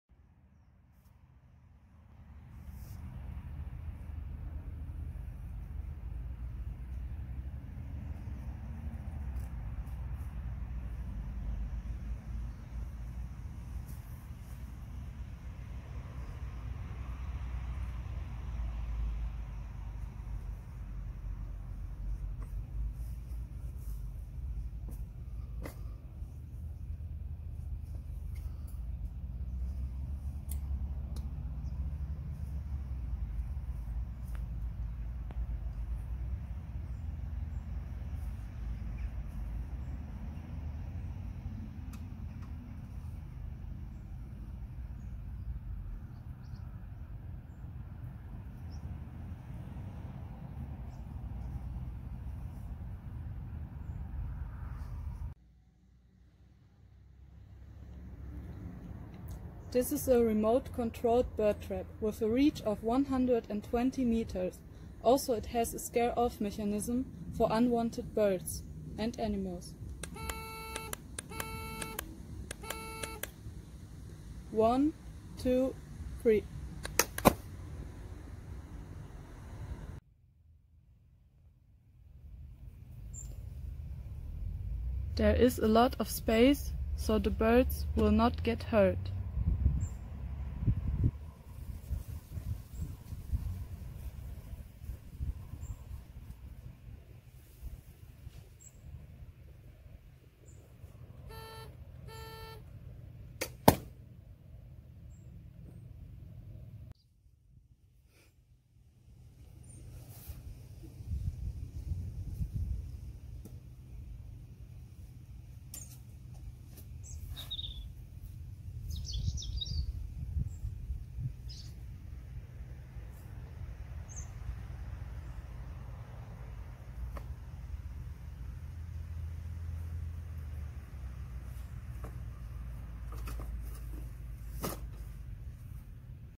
Zusammenklappbare Vogelfalle mit Fernauslösung, bird trap, Schlagnetz, Taubenfalle, Bodenfalle
Eine Vogelfalle, die sich zusammenklappen lässt.
In zwei Größen erhältlich:
80cm × 80cm (zusammengeklappt: 40cm × 20cm)
100cm × 100cm
(zusammengeklappt: 50cm x 25cm)
Bedient wird die Vogelfalle über eine Fernbedienung die eine Reichweite von mindestens 120 Meter besitzt.

Will teilen: Vogelfalle einklappbar Bird Trap Lebendfalle Stieglitz Kanarien Schlagnetz